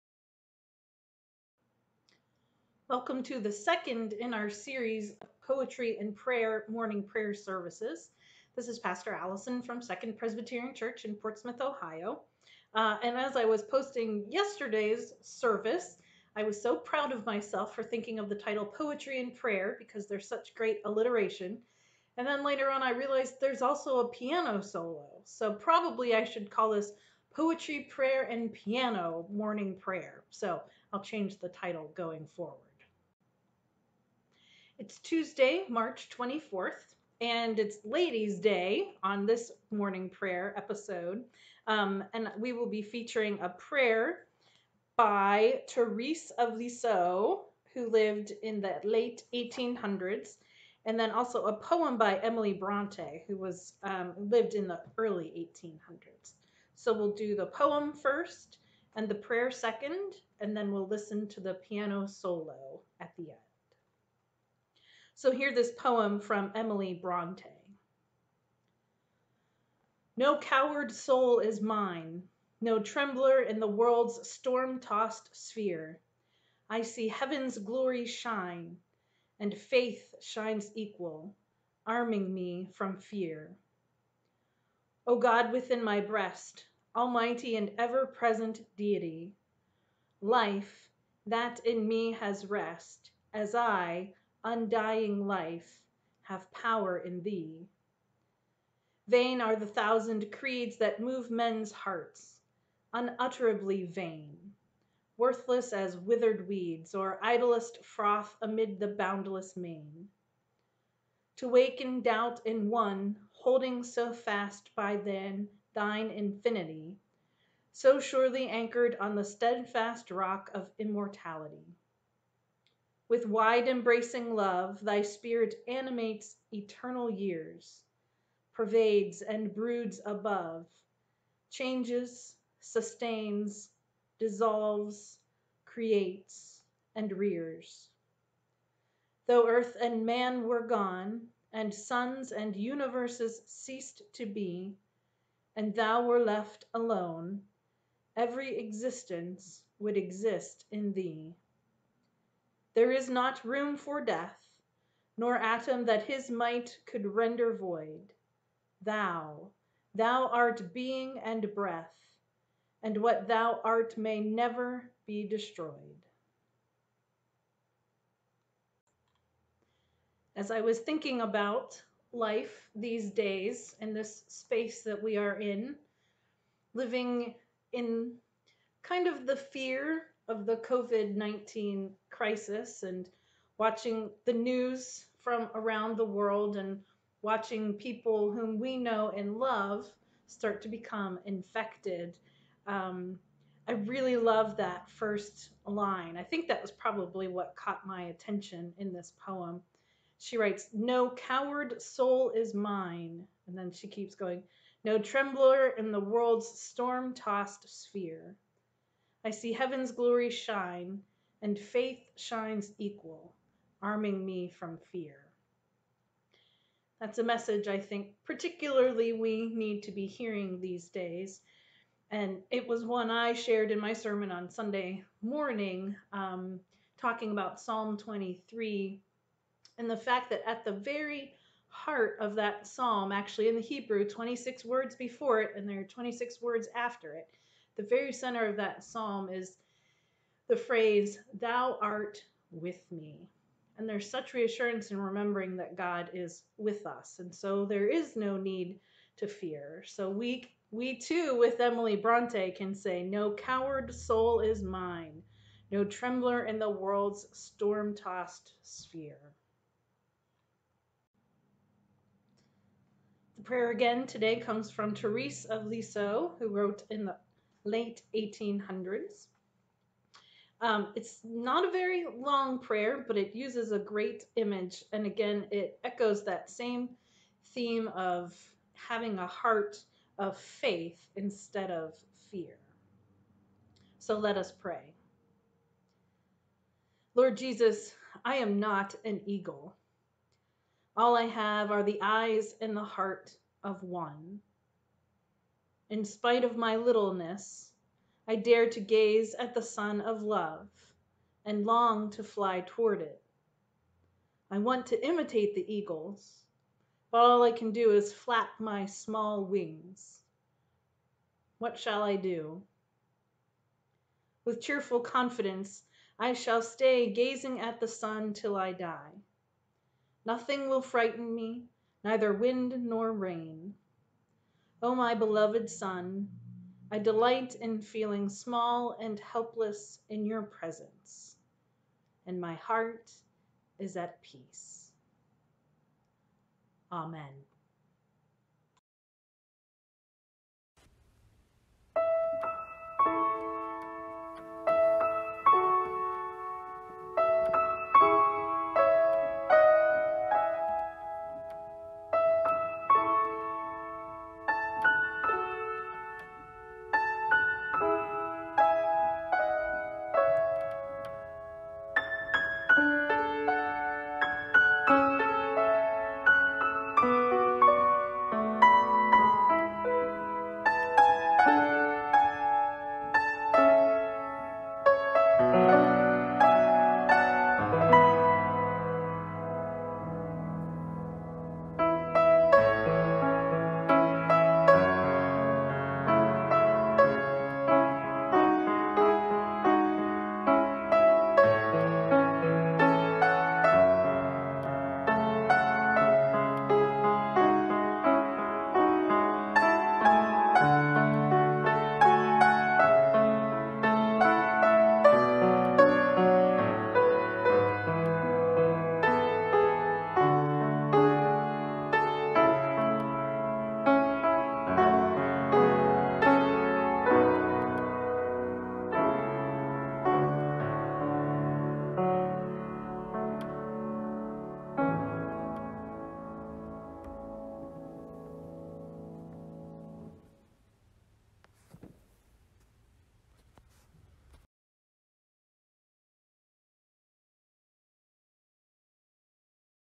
Tues, March 24 Morning Prayer
"Poetry, Prayer & Piano" morning prayer service. stayingconnected through worship during the COVID-19 crisis.